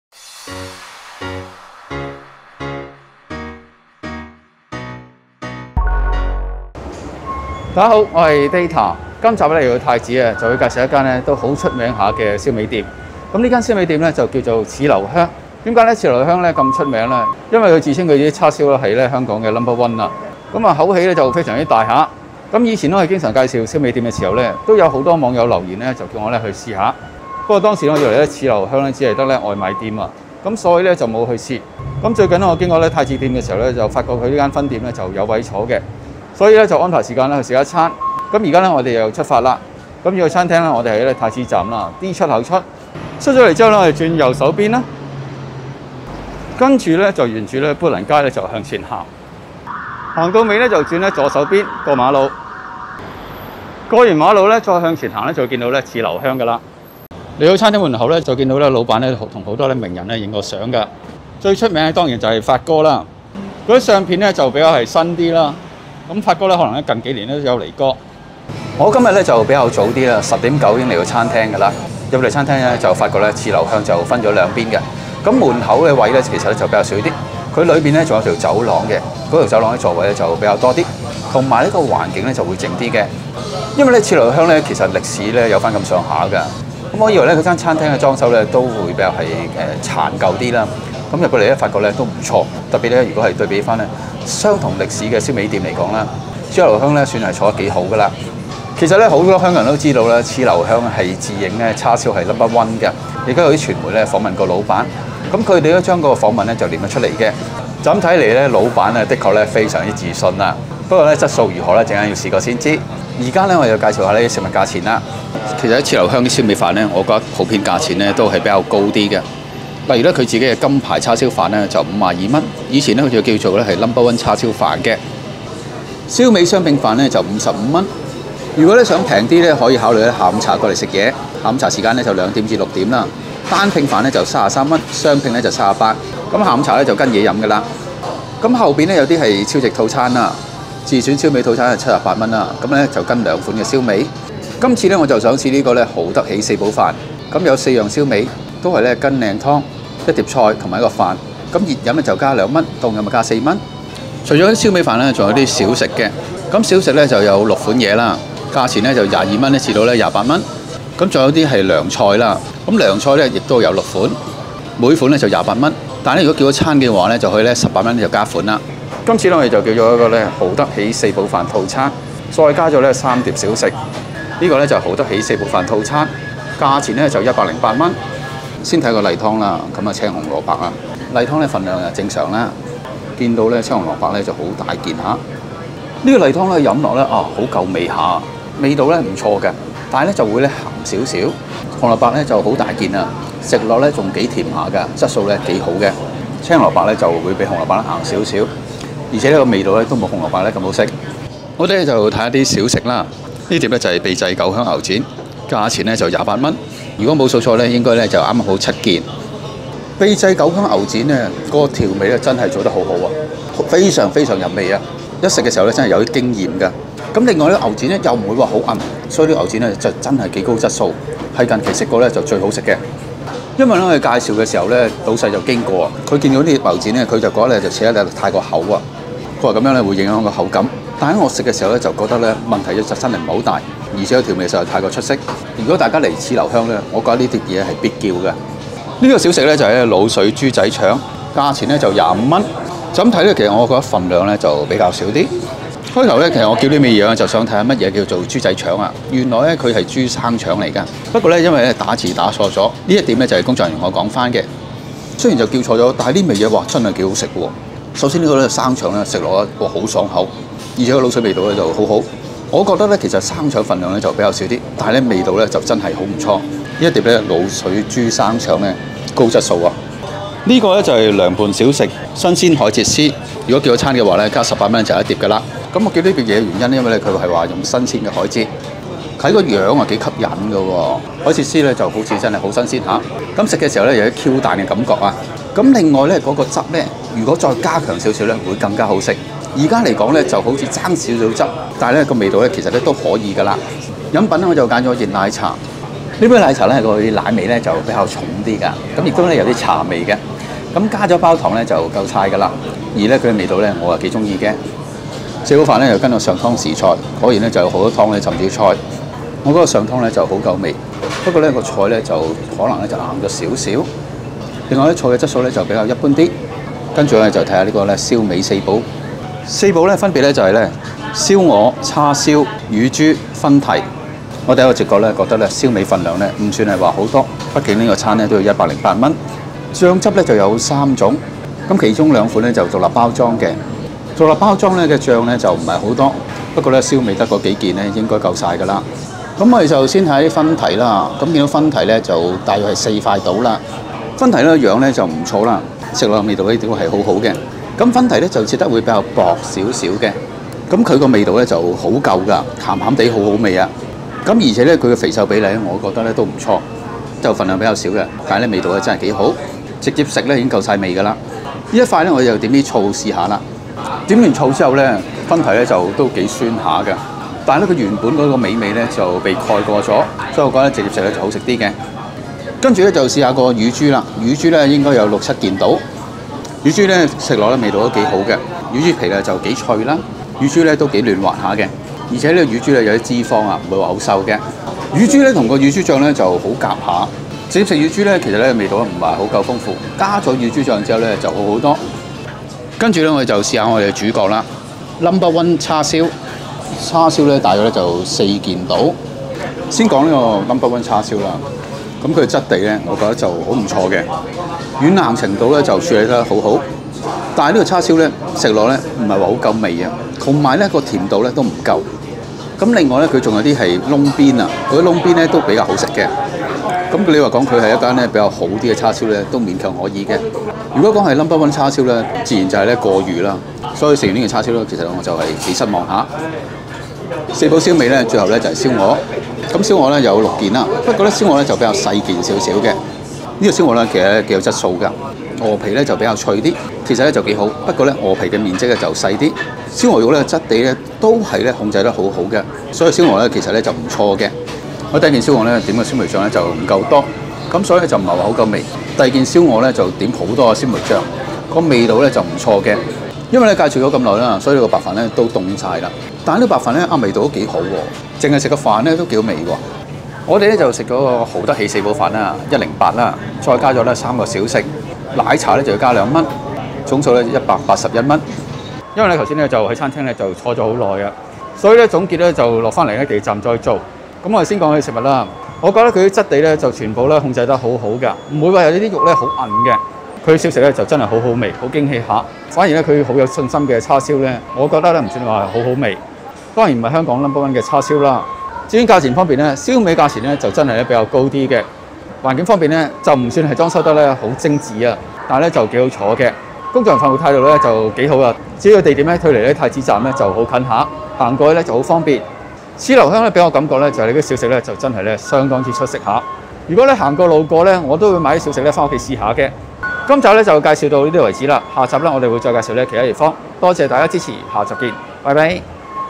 [神州穿梭.香港#642] 自稱香港第一叉燒 發哥都有光顧 小食非常出色 令人驚豔 秘製九香牛展 非食不可 太子 | 齒留香
各位網友，我在Bilibili(B站)也有頻道

齒留香
地址: 太子界限街26號地舖
港鐵太子站 D 出口, 步行約3分鐘
電話: 26237879